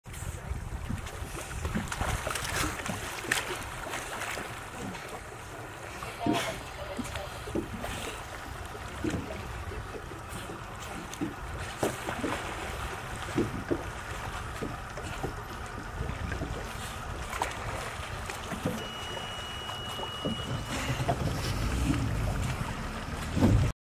California Sea Lion Colony - Isla Calavera, Bahia de Los Angeles - Baja California
Colony of California Sea Lions filmed in February 2014 at Bahia de Los Angeles - Baja California Mexico